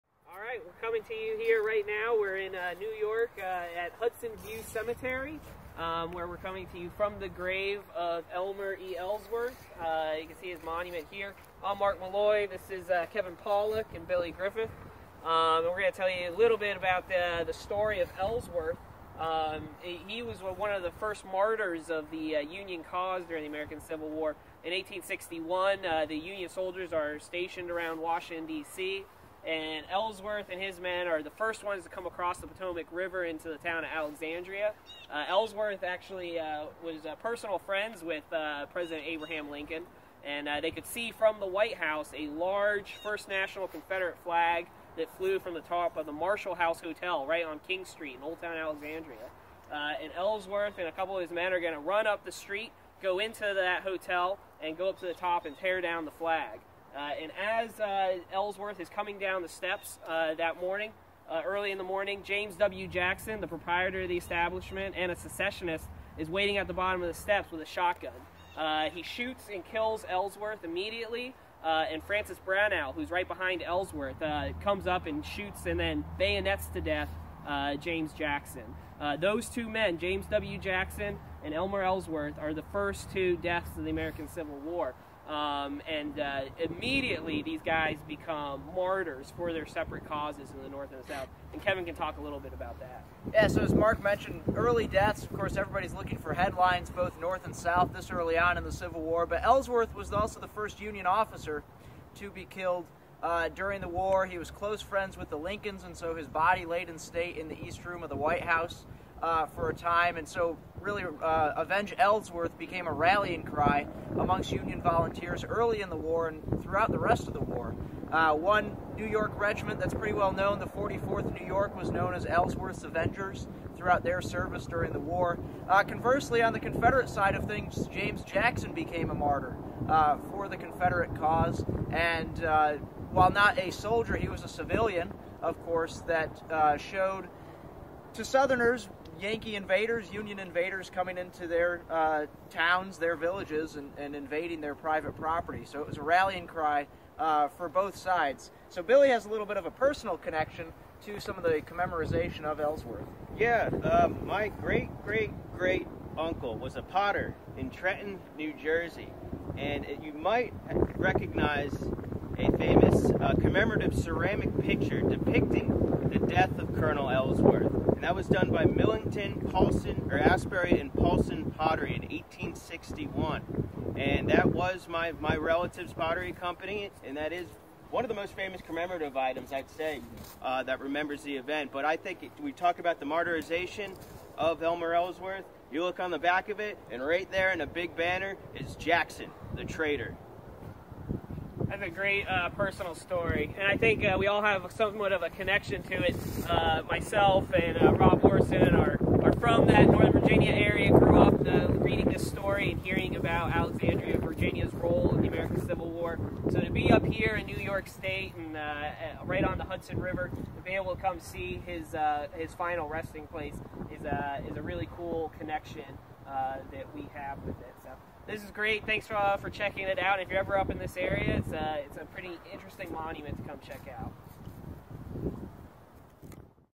At the Grave of Elmer Ellsworth
Emerging Civil War Historians Mark Mallow, Kevin Pawlak, and Billy Griffith visit the grave of Elmer Ellsworth. Was he the north's first martyr or was he a trespasser who deserved to get shot?